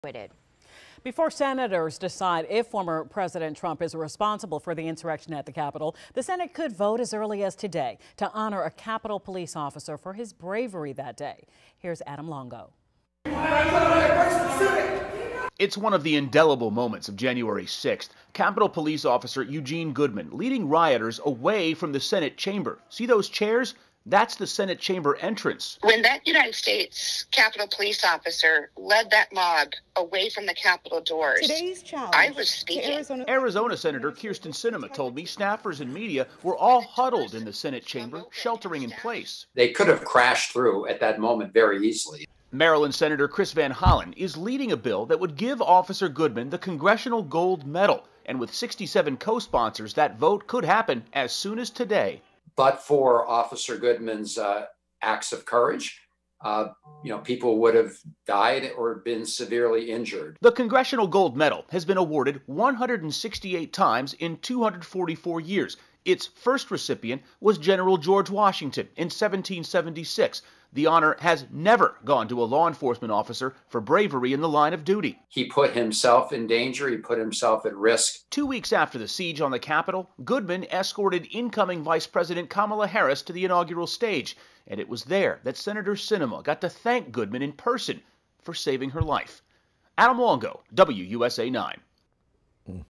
Officer Eugene Goodman, hero at Capitol riots, considered for Congressional Gold Medal
Goodman led a portion of the pro-Trump mob away from the Senate chambers where lawmakers were still evacuating. Now he's up for a Congressional medal.

We're tracking all of the arrests, charges and investigations into the January 6 assault on the Capitol. Sign up for our Capitol Breach Newsletter